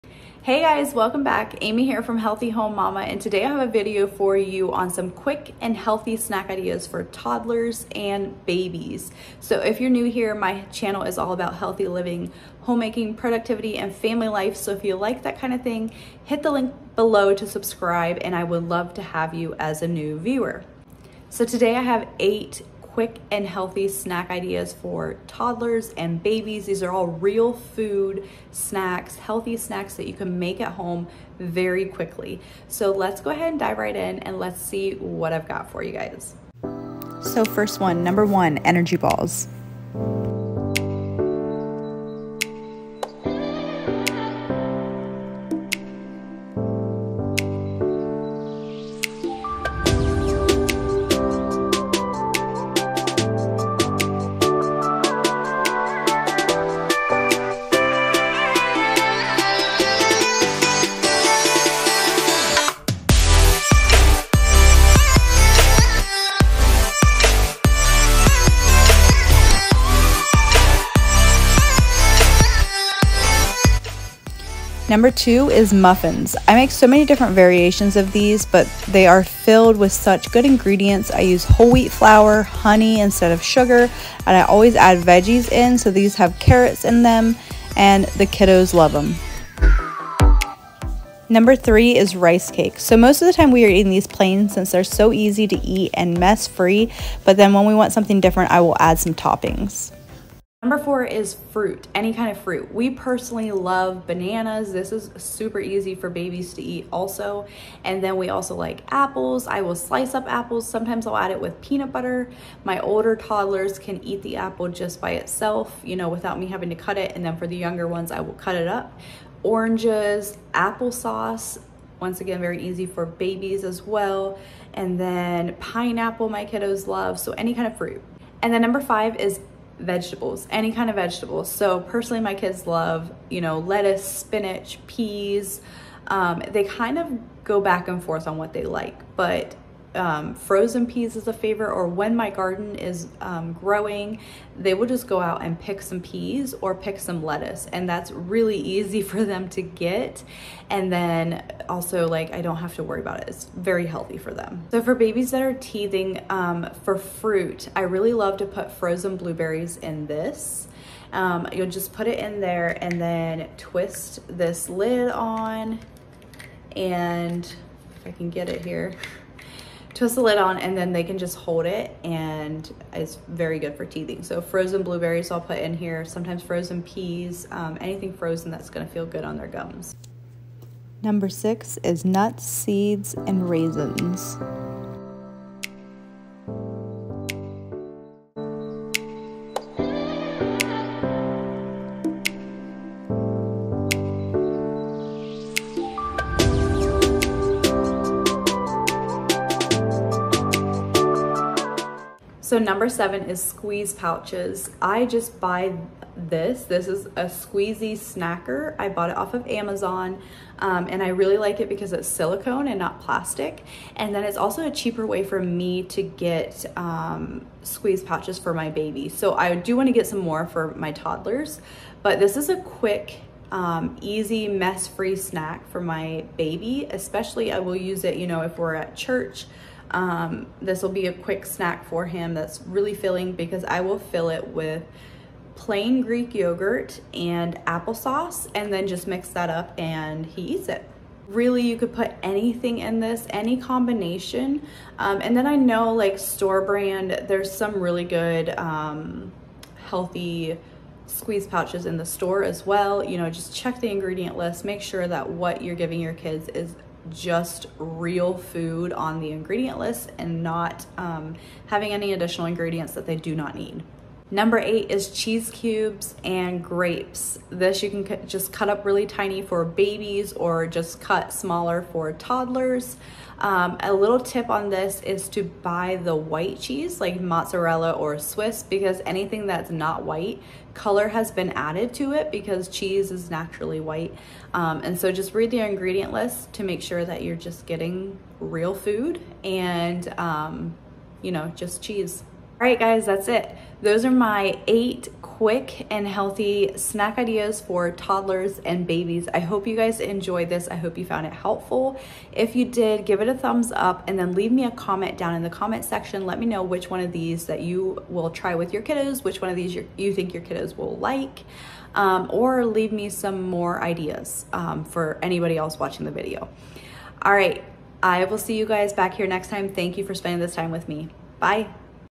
8 QUICK AND HEALTHY SNACK IDEAS FOR TODDLERS & BABIES (real food snacks)
These 8 quick and healthy snack ideas are all real food snacks for toddlers and babies. Feel good giving your toddlers and babies these snacks knowing they will be nourishing their growing bodies and minds.